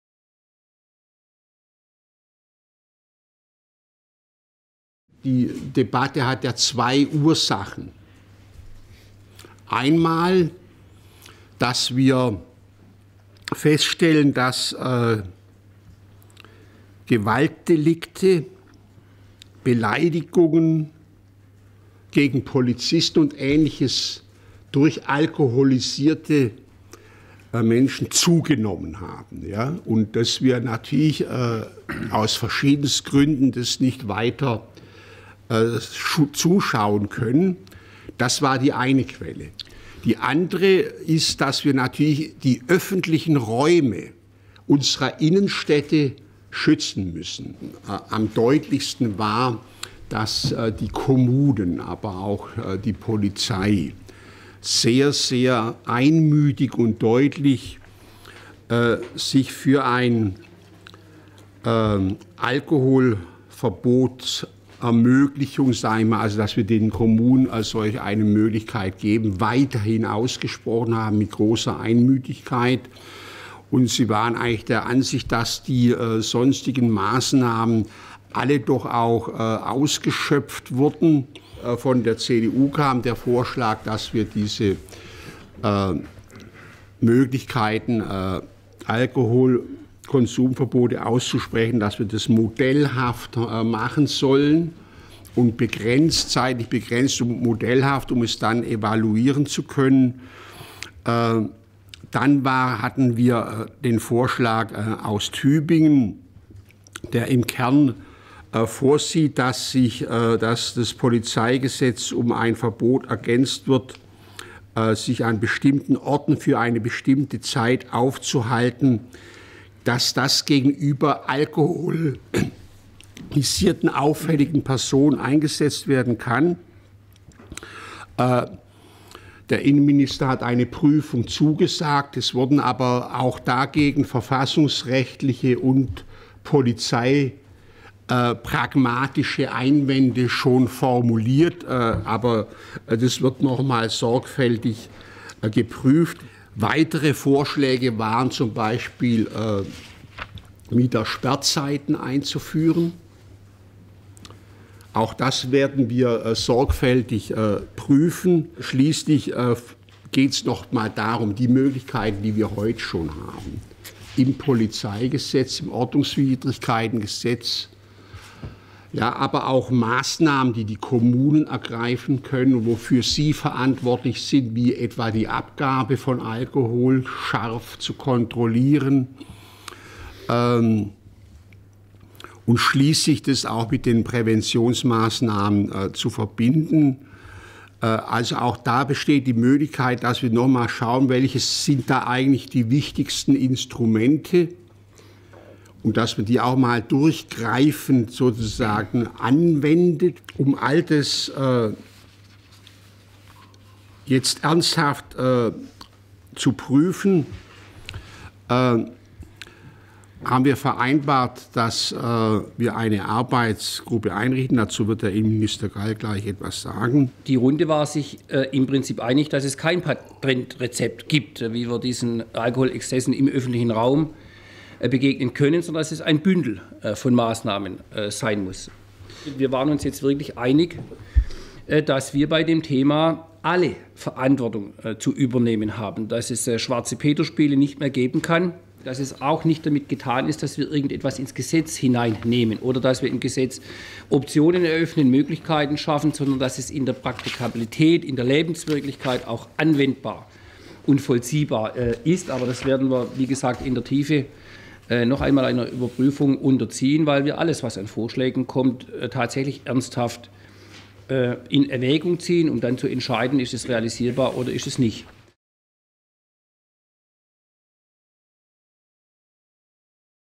Pressekonferenz zum Runden Tisch für einen lebenswerten öffentlichen Raum
In verschiedenen Städten des Landes kommt es regelmäßig zu Problemen in Zusammenhang mit übermäßigem Alkoholkonsum auf öffentlichen Plätzen. Ministerpräsident Winfried Kretschmann hatte deshalb zu einem Runden Tisch für einen lebenswerten öffentlichen Raum  eingeladen, an dem neben der Landesregierung Vertreterinnen und Vertreter von Polizei, Kommunen, Wissenschaft, Verbänden, Parteien und Landtagsfraktionen teilnahmen. Die Runde einigte sich darauf, eine Arbeitsgruppe einzurichten, die mögliche Gegenmaßnahmen gegen Alkoholexzesse diskutieren und Vorschläge entwickeln soll.